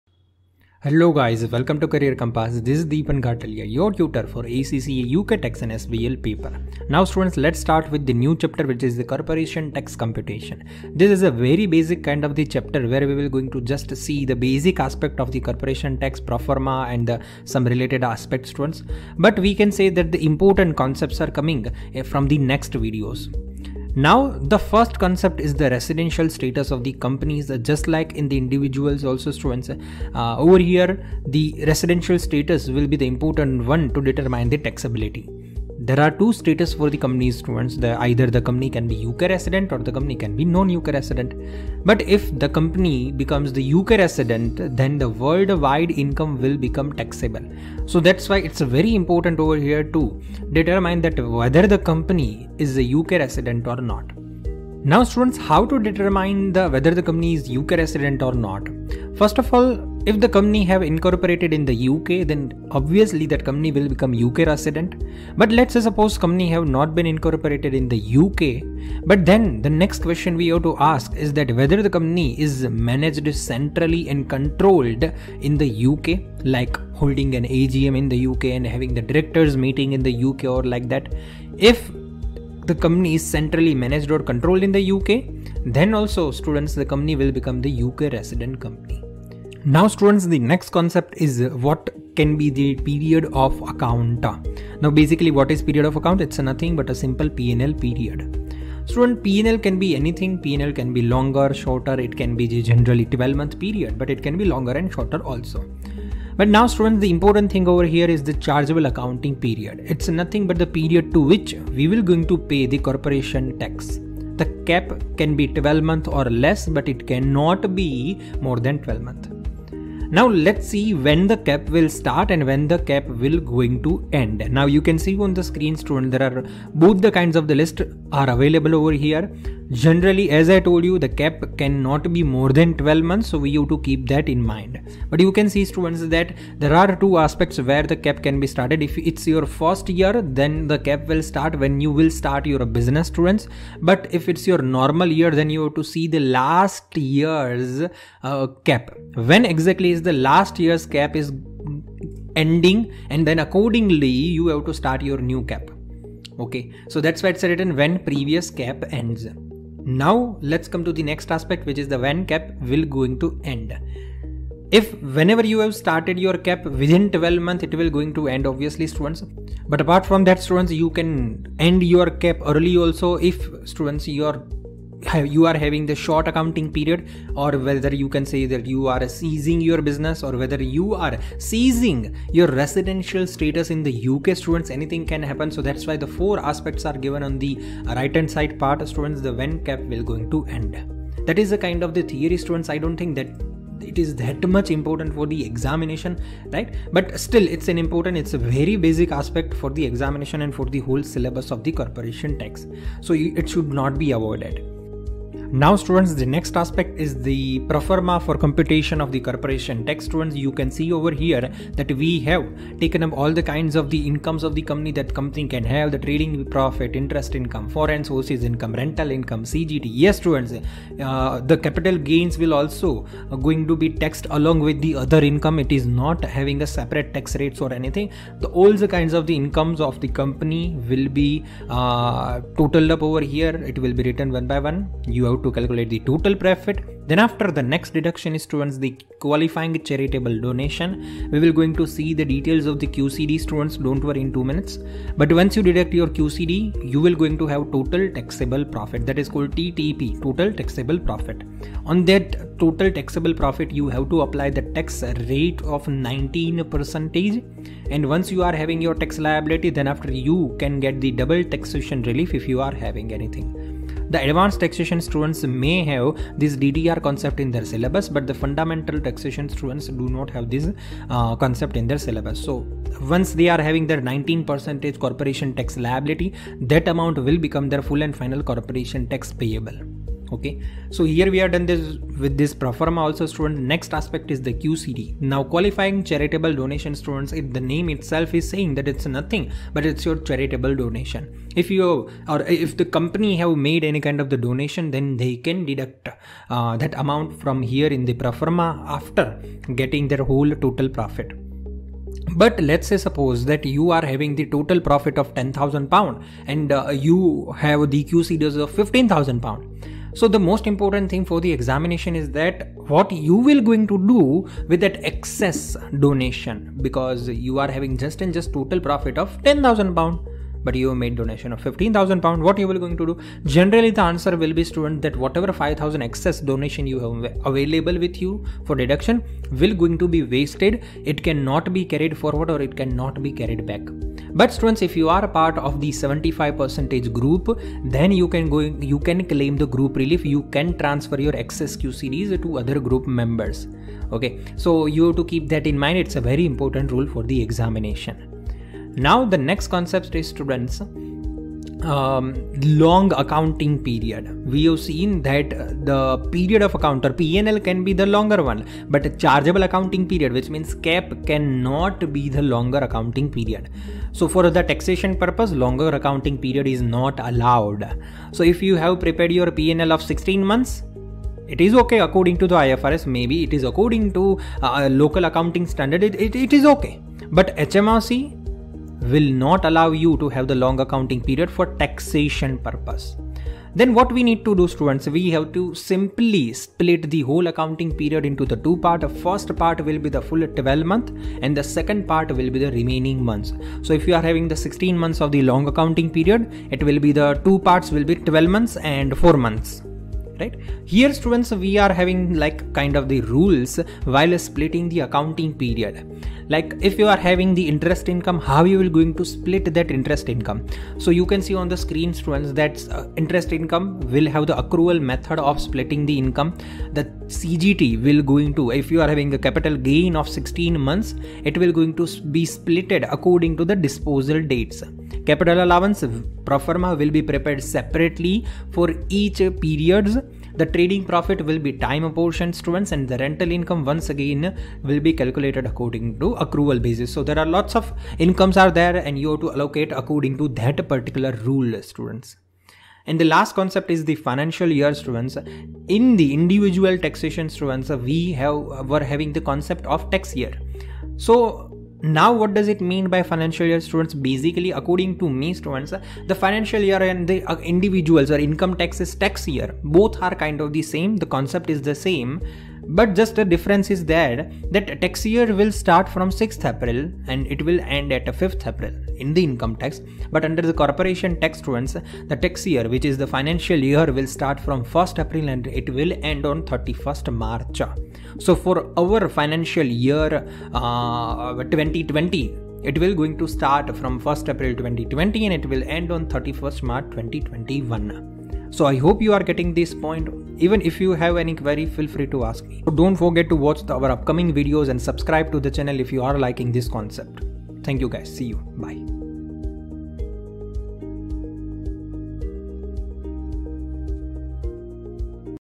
Rapid Revision | ACCA TX/ATX | Chapter 14 - Corporation Tax Basics | FA20 | By Dipan Ghataliya
Rapid revision of ACCA UK Taxation exam Chapter 14 - Corporation Tax Basics (FA20) By Dipan Ghataliya.

About this video:

Hello guys, This is Dipan Ghataliya, Tutor at Career Compass for UK Tax and SBL Subject.

In this video, I tried to revise the whole chapter of Income Tax computation very quickly for end moment revision before the ACCA exam days. But this video should not be used to understand the basic fundamentals of UK Taxation.

I tried my best to provide a very rapid and quick but still detailed conceptual clarity but still if you have any query regarding ACCA UK Taxation feel free to contact me personally.

0:00 - Introduction
0:23 - Residential status
1:30 - Period of account
1:48 - Chargeable accounting period
3:25 - Corporation Tax Computation
4:43 - QCD
5:48 - Long period of accounting
7:22 - Financial Year